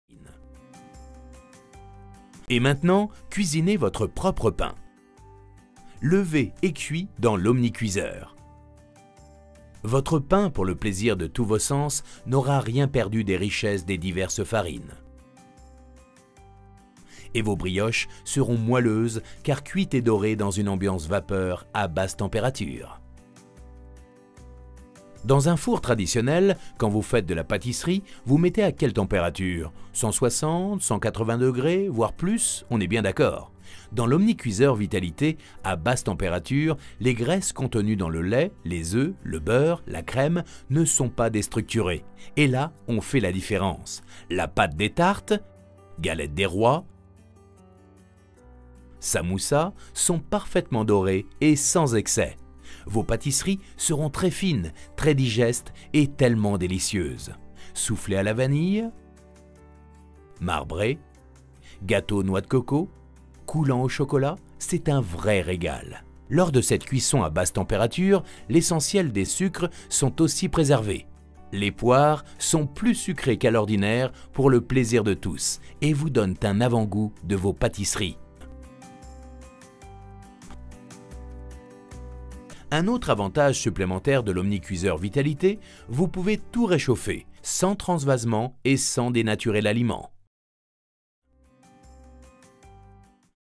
Cuisiner gâteaux, pain tartes à l'Omnicuiseur Vitalité
L'Omnicuiseur Vitalité, appareil de cuisson basse température alliant infrarouges et cuisson vapeur, il permet de tout cuisiner facilement au quotidien, gâteaux, pain, tartes...

Cuisiner devient un véritable plaisir et les repas une fête au quotidien.
Le secret : notre cuisson basse température

Découvrez nos animations et dégustations dans les magasins bio ou sur notre site internet